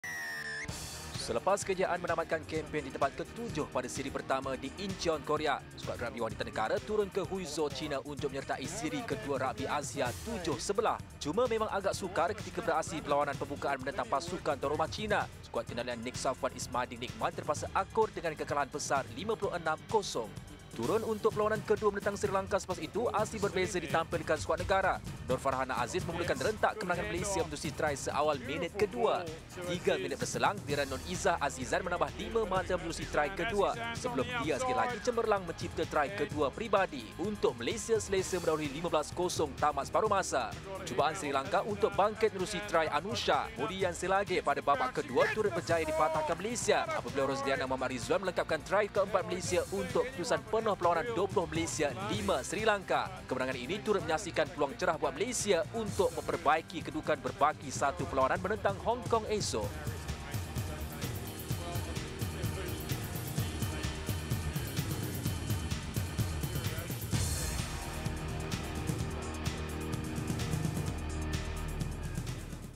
RAGBI | Malaysia Atasi Sri Lanka
Skuad ragbi wanita negara memulakan aksi pada Siri Ragbi Asia 7 Sebelah di Huizhou, China dengan langkah kanan.

Walaupun terpaksa akur tewas kepada pasukan tuan rumah, China pada aksi pembukaan, persembahan cemerlang pada perlawanan kedua membolehkan skuad negara merakam kemenangan dengan mengatasi Sri Lanka.